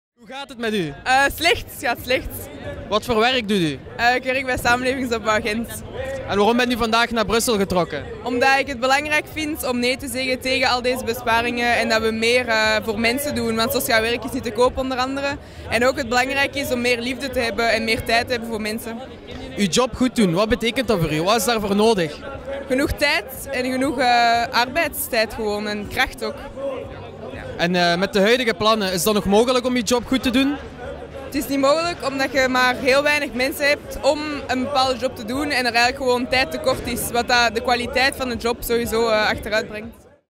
Stephanie (Samenlevingsopbouw Gent) wil minder werkdruk om betere zorg te kunnen geven
Op de nationale vakbondsbetoging van 29 september 2016 liepen we Stephanie tegen het lijf. Zij werkt bij Samenlevingsopbouw Gent en betoogde mee voor minder werkdruk. Dat zou de kwaliteit van de zorg ten goede komen.